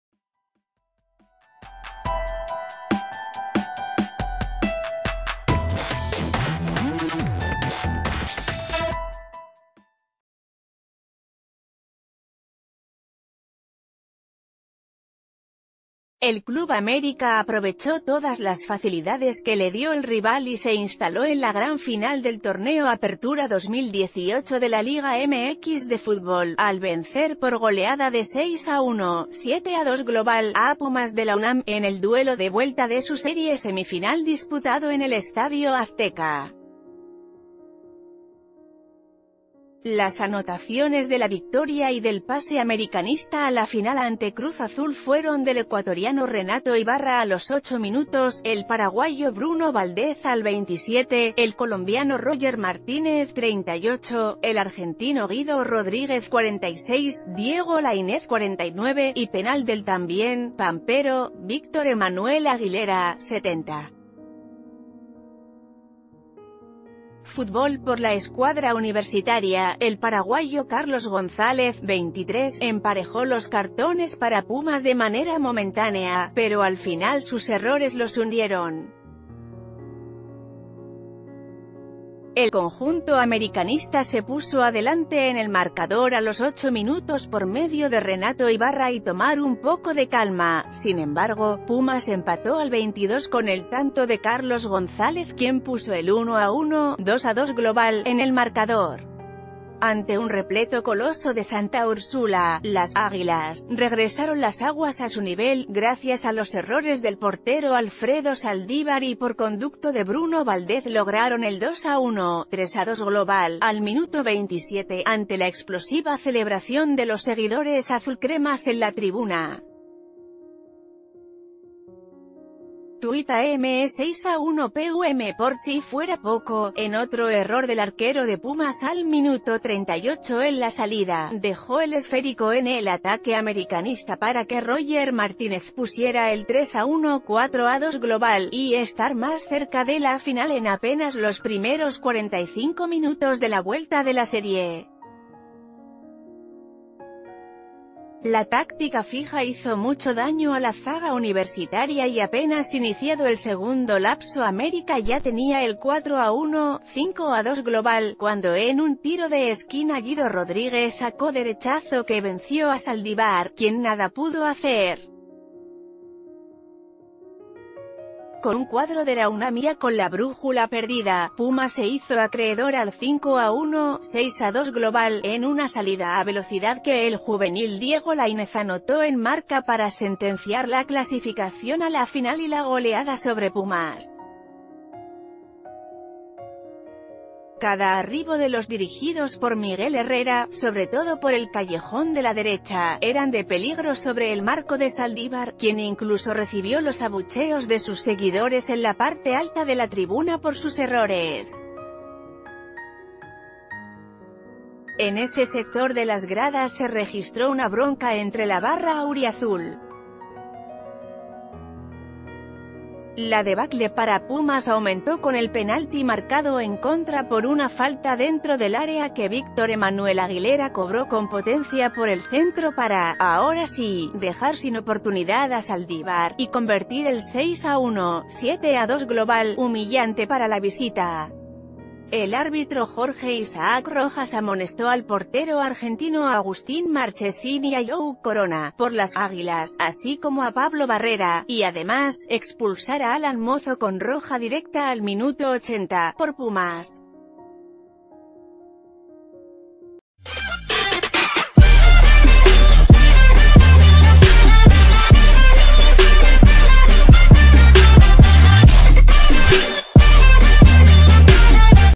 América humilla 6-1 a Pumas y está en la final del Apertura 2018
El Club América aprovechó todas las facilidades que le dio el rival y se instaló en la gran final del Torneo Apertura 2018 de la Liga MX de futbol, al vencer por goleada de 6-1 (7-2 global) a Pumas de la UNAM, en el duelo de vuelta de su serie semifinal disputado en el estadio Azteca. Las anotacione...